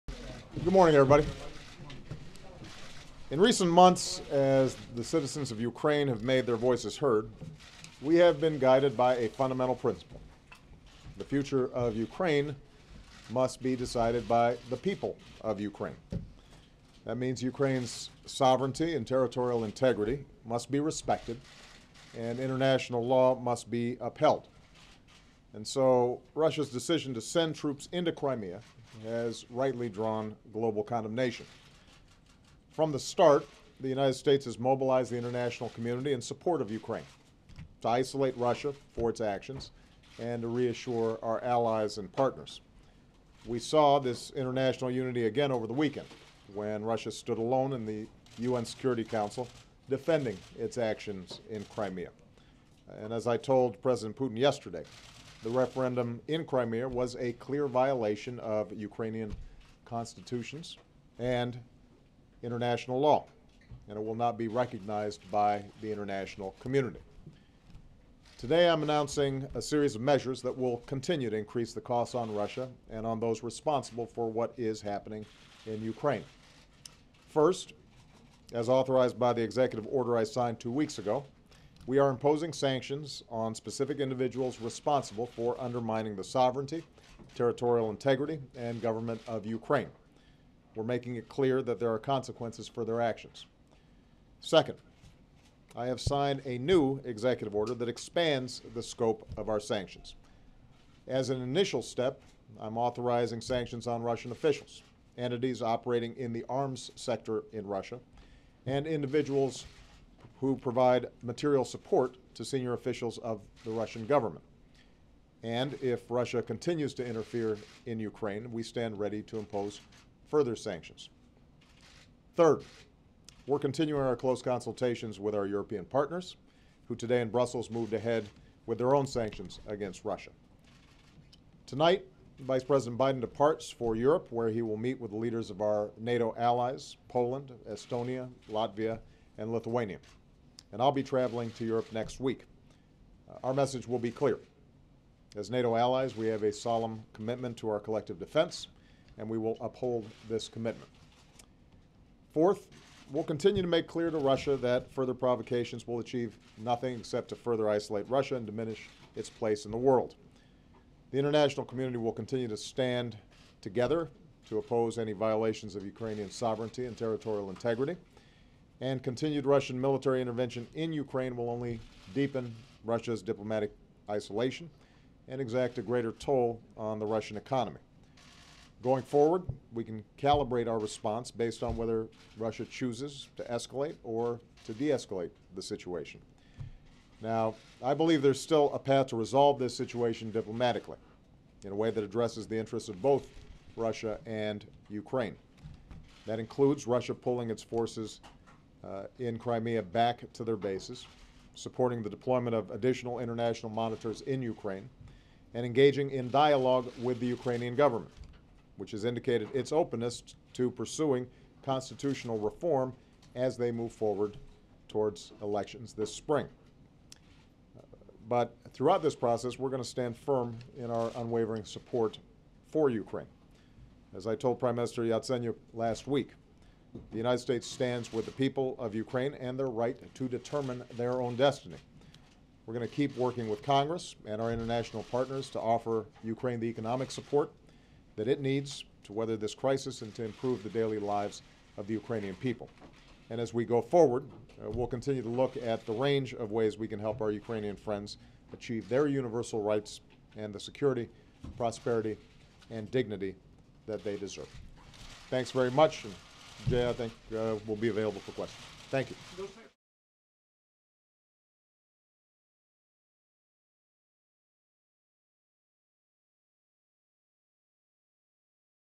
President Obama Speaks on Ukraine
In a statement from the White House Press Briefing Room, President Obama announces a series of measures that will continue to increase the cost on Russia and on those responsible for what is happening in Ukraine. March 17, 2014.